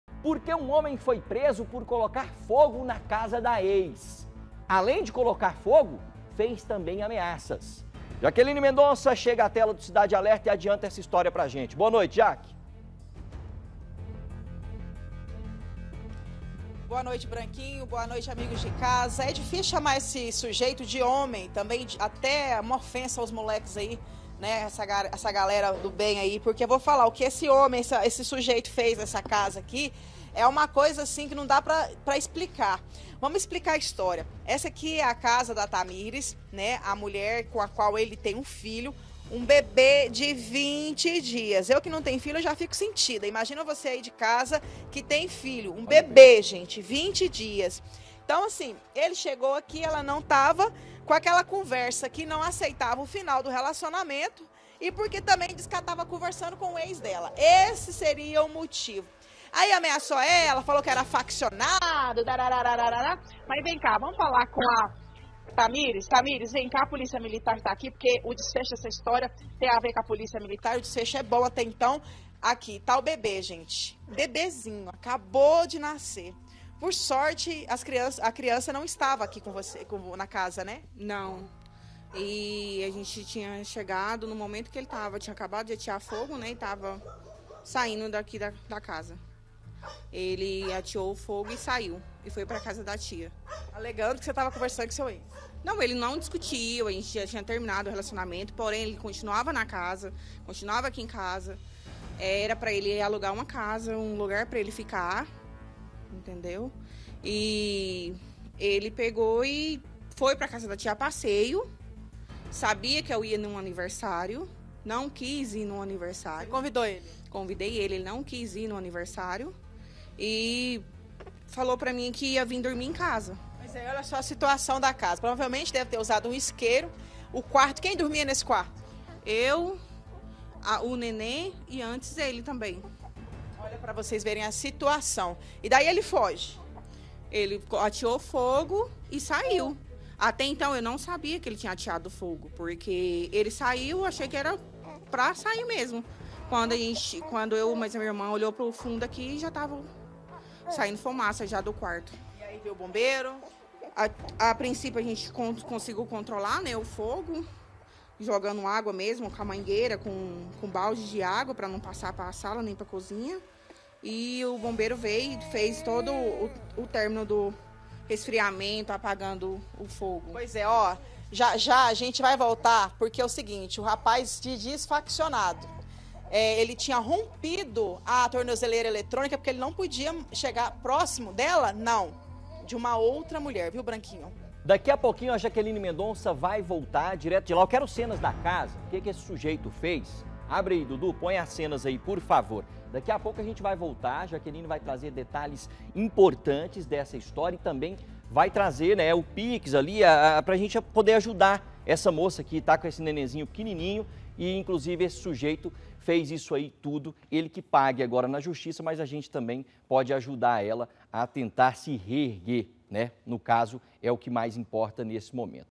ESTAVA COM CIÚMES: HOMEM COLOCA FOGO NA CASA DA EX
HOMEM É PRESO POR COLOCAR FOGO NA CASA DA EX-MULHER E FAZER AMEAÇAS.